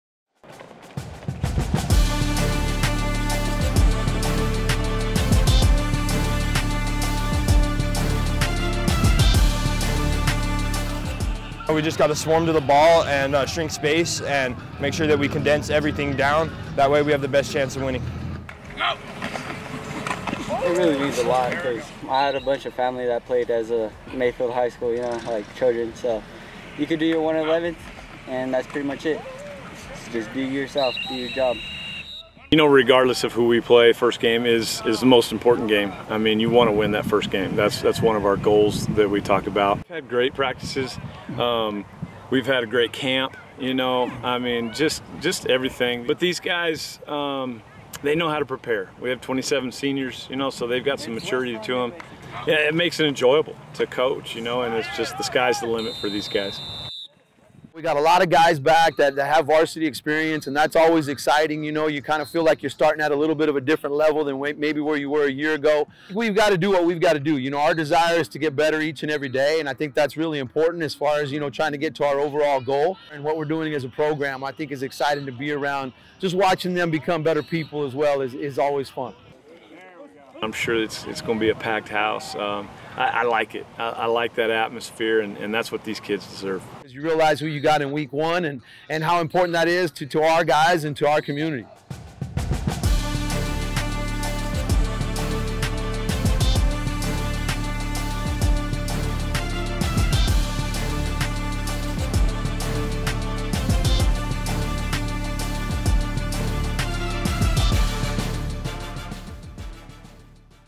Bulldawgs vs Trojans football preview
The 2024 high school football season at Field of Dreams kicks off with a heated rivalry game - Las Cruces Bulldawgs vs. Mayfield Trojans.
Tune into the live stream action Friday, August 23 at 7 pm only on LCPS.TV.